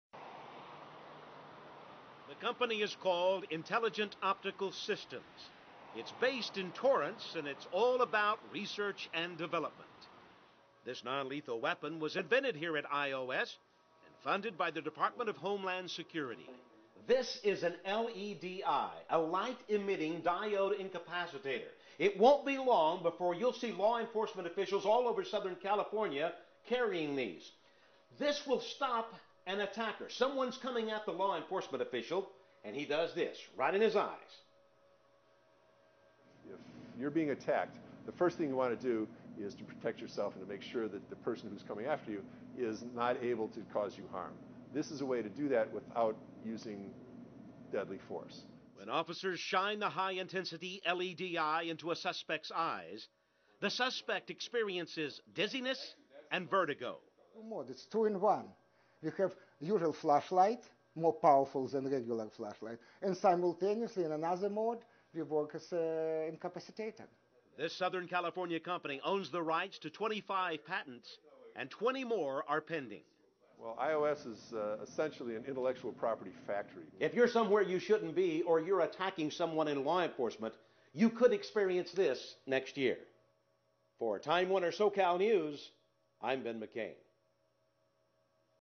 IOS "Puke" Flashlight
LED incapacitator flashlight from Intelligent Optical Systems.

Make your own!